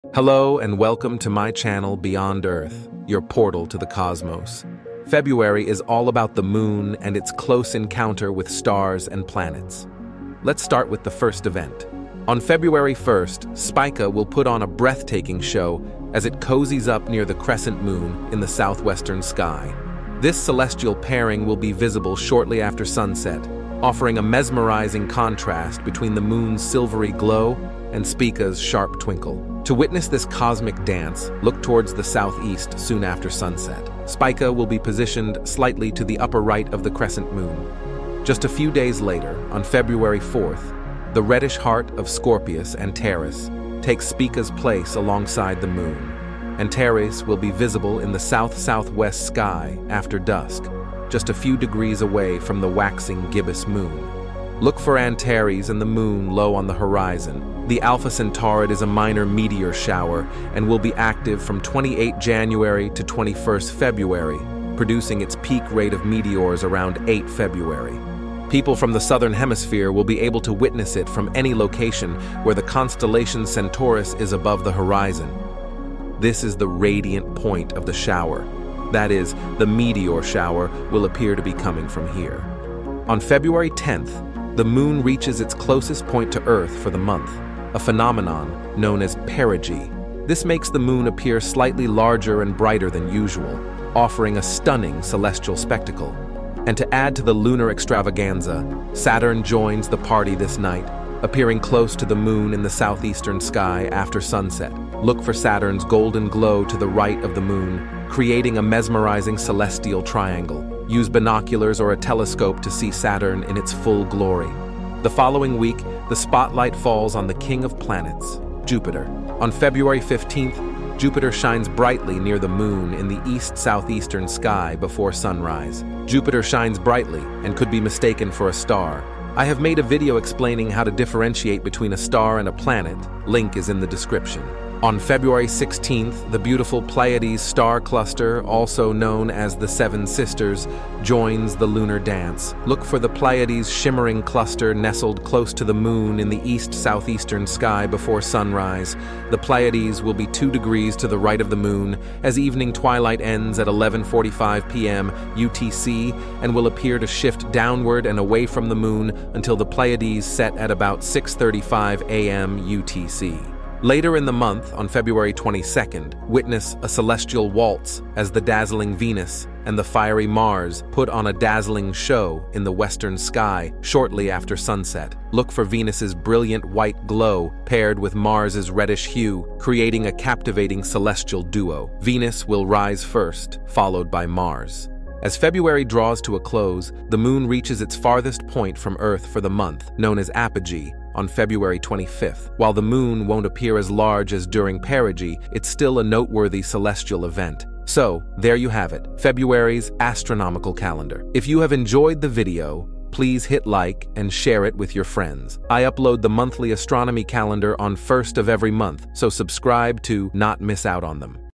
Must Watch ASTRONOMY Events in FEBRUARY 2024 | Moon at Perigee | Stars | Planets | Beyond Earth
Gear up for a month of cosmic wonders in February! From dazzling meteor showers to planetary pairings and a super close Moon, the night sky is putting on a spectacular show. This video is your ultimate guide to February's astronomical events, so grab your star charts and get ready to be amazed!

Here's a glimpse of what you'll discover:

Witness the Alpha Centaurids meteor shower paint the night sky with streaks of light.
Marvel at Venus and Mars making a close encounter.
Be awestruck by the Moon reaching its closest and farthest points to Earth.
Learn about Spica, Antares, and other celestial stars putting on a stellar performance.

So don't miss out on this incredible celestial happening!

Timeline:
00:00 Intro
00:12 Spica near Moon
00:38 Antares near Moon
00:57 Alpha Centaurids meteor shower
01:23 Moon at closest
01:36 Saturn near Moon
01:54 Jupiter near Moon
02:14 Pleiades star cluster near Moon
02:43 Mars Venus together
03:05 Moon at farthest
03:17 Outro

February astronomy, meteor showers, Alpha Centaurids, planets, Mars, Jupiter, Venus, Moon, perigee, apogee, constellations, Spica, Antares, naked eye astronomy, observing tips